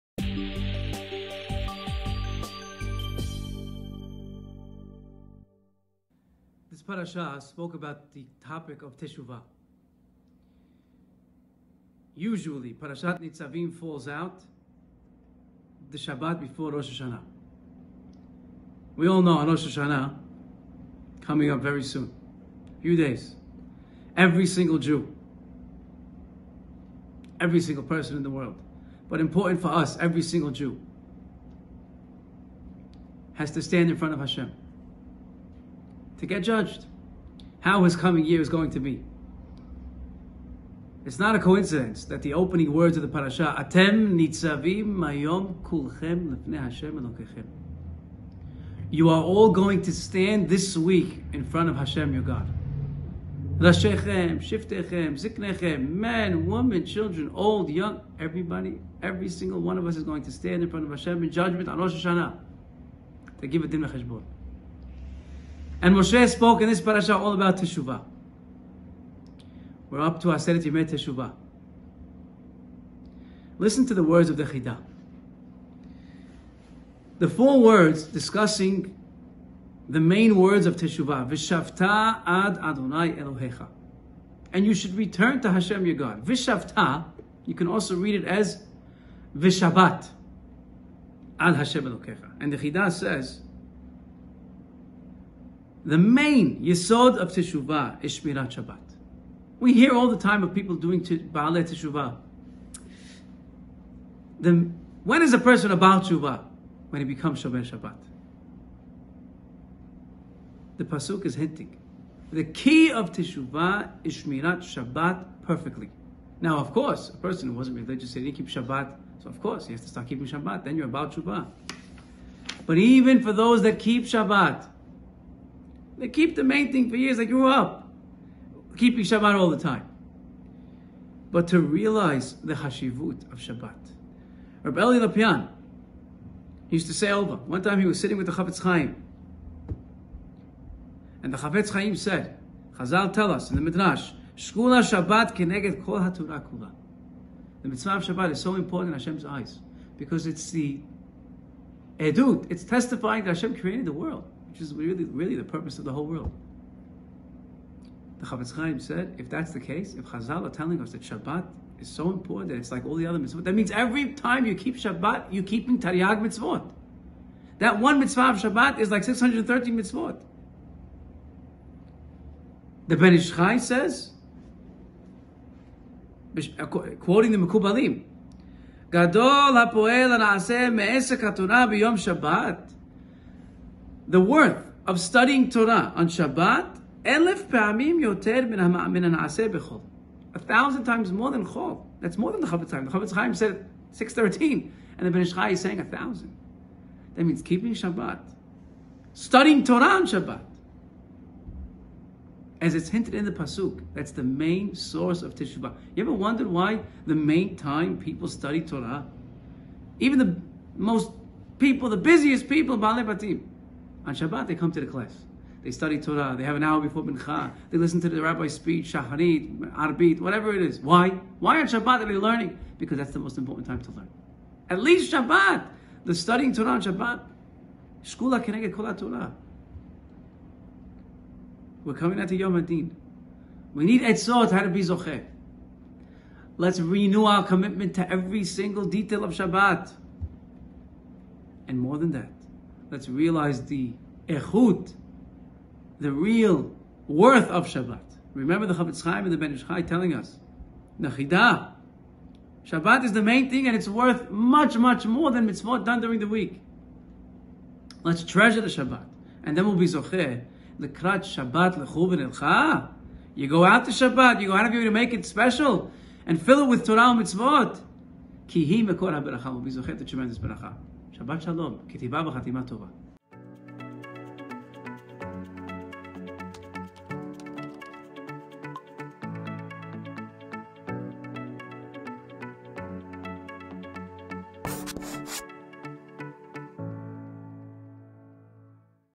Shnayim Yomi - Nitzavim - D'var Torah
Rabbi Eliyahu Aboud wraps up this weeks Shnayim Mikra with a beautiful lesson.

Thank you Rabbi Aboud for carrying us though the week with your clear explanation!

Shabbat Shalom!